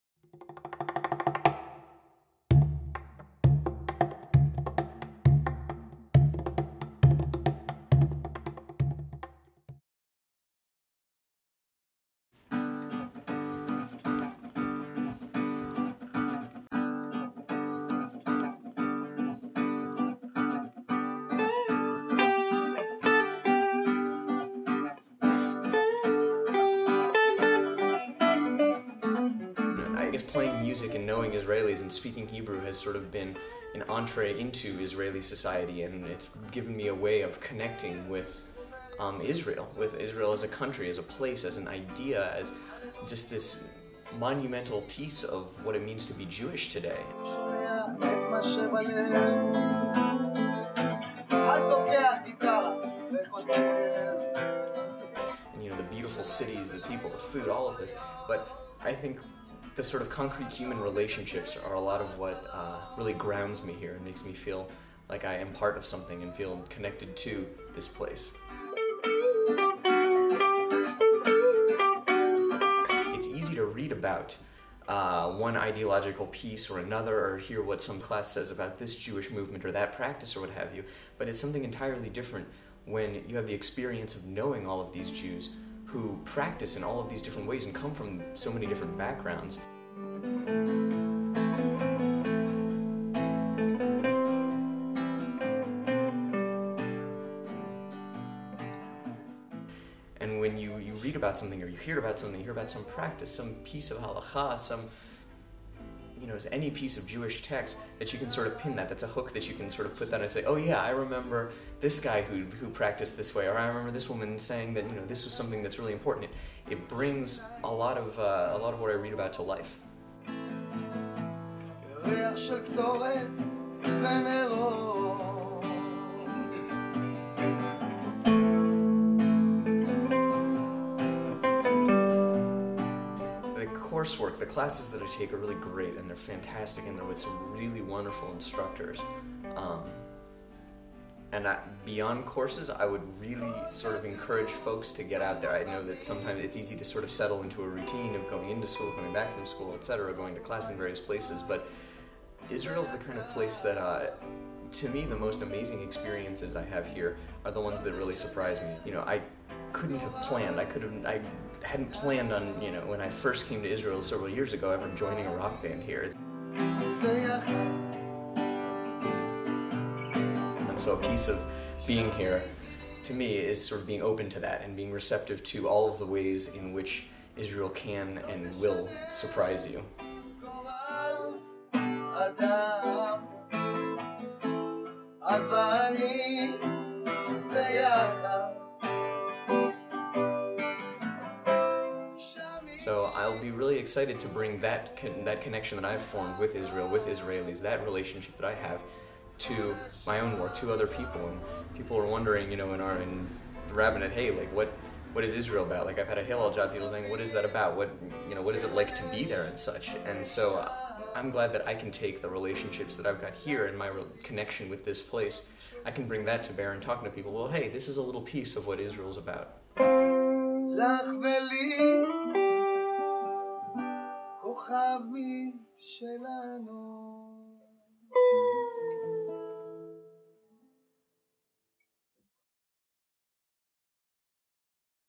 Nick: Connecting to Israel Through Music, People
Nick Renner describes how music bonds him with Israel, land of surprises. Concert excerpts also. More at RRC.edu